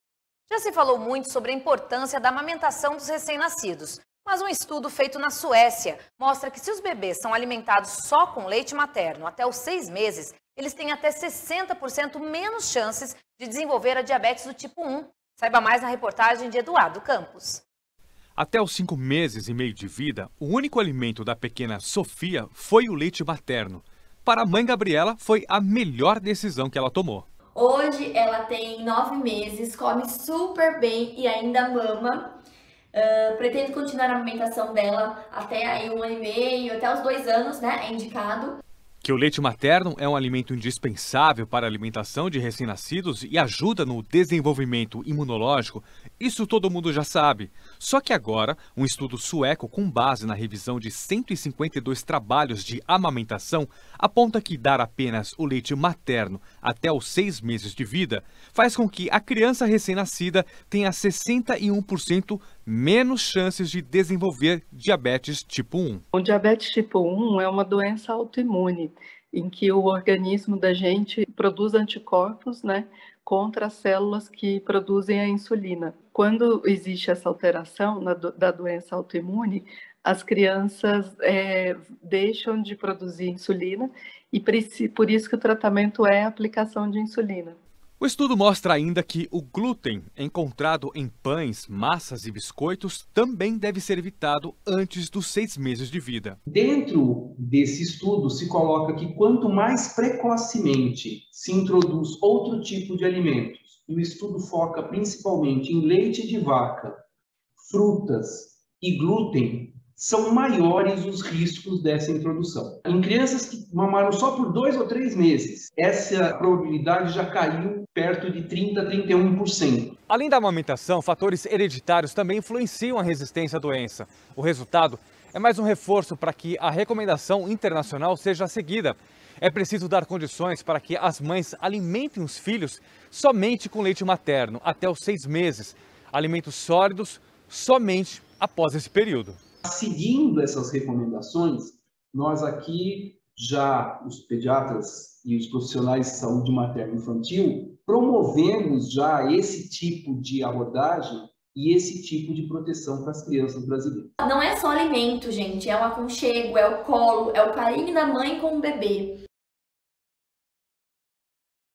VT AMAMENTAÇÃO DIABETES
JÁ SE FALOU MUITO SOBRE A IMPORTÂNCIA DA AMAMENTAÇÃO DOS RECÉM NASCIDOS. MAS UM ESTUDO FEITO NA SUÉCIA MOSTRA QUE SE OS BEBÊS SÃO ALIMENTADOS  SÓ COM O LEITE MATERNO ATÉ OS SEIS MESES , ELES TEM ATÉ SESSENTA POR CENTO MENOS CHANCES DE DESENVOLVER A  DIABETES DO TIPO UM.  SAIBA MAIS NA REPORTAGEM DE EDUARDO CAMPOS.